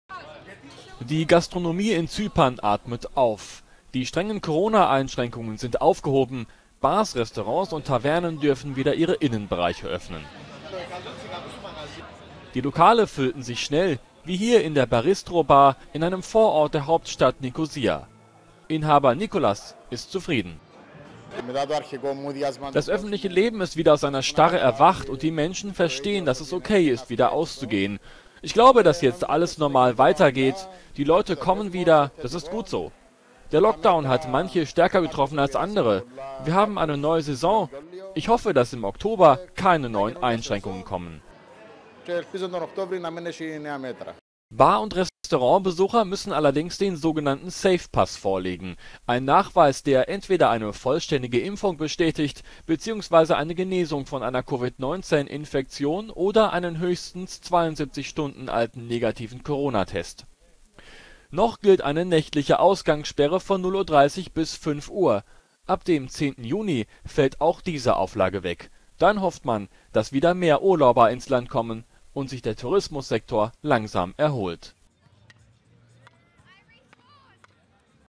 Zypern lockert wieder: Menschen strömen in die Bars
Zypern lockert wieder: Bars und Restaurants dürfen wieder ihre Innenbereiche öffnen. Besucher:innen müssen allerdings den sogenannten "SafePass" vorlegen.